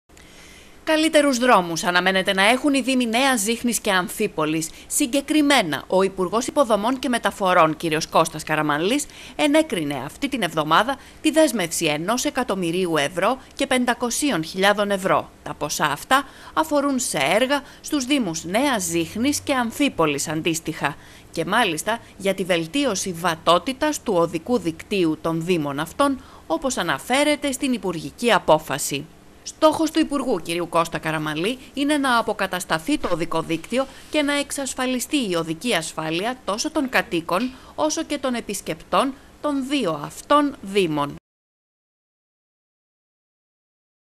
Καραμανλής: 1,5 εκατ. ευρώ για την βελτίωση του οδικού δικτύου σε Νέα Ζίχνη και Αμφίπολη
Καλύτερους δρόμους αναμένεται να έχουν οι δήμοι Νέας Ζίχνης και Αμφίπολης.